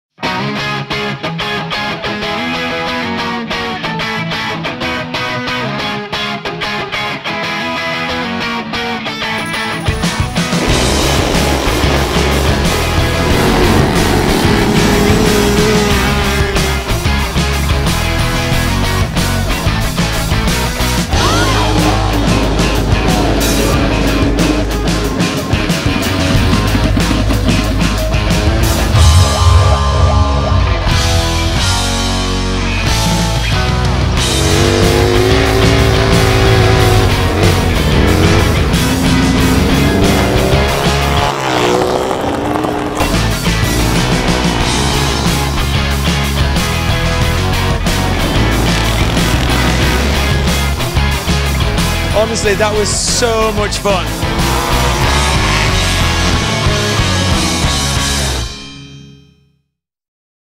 Indian in action at the El Rollo flat track race 2018
Indian at El Rollo - it was dusty, dirty and real fast 😜